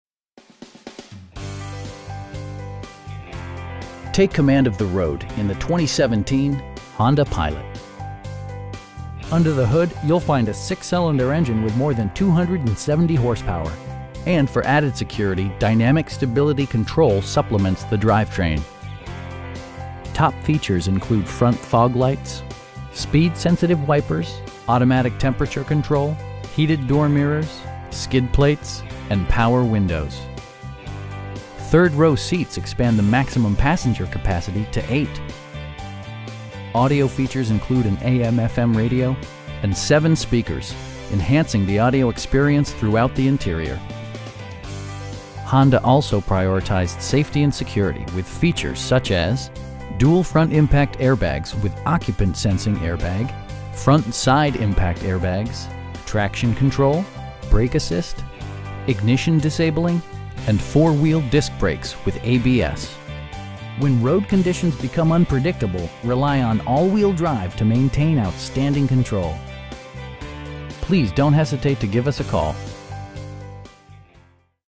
2017 Honda Pilot EX in Cary, NC 27511-7910
Hendrick Dodge Ram FIAT

Take command of the road in the 2017 Honda Pilot. Under the hood you'll find a 6 cylinder engine with more than 270 horsepower, and for added security, dynamic Stability Control supplements the drivetrain. Top features include front fog lights, speed sensitive wipers, automatic temperature control, telescoping steering wheel, heated door mirrors, skid plates, an overhead console, and power windows. Third row seats expand the maximum passenger capacity to 8. Audio features include an AM/FM radio, and 7 speakers, providing excellent sound throughout the cabin. Honda also prioritized safety and security with features such as: dual front impact airbags with occupant sensing airbag, front side impact airbags, traction control, brake assist, ignition disabling, and 4 wheel disc brakes with ABS. When road conditions become unpredictable, rely on all wheel drive to maintain outstanding control. We know that you have high expectations, and we enjoy the challenge of meeting and exceeding them! Please don't hesitate to give us a call.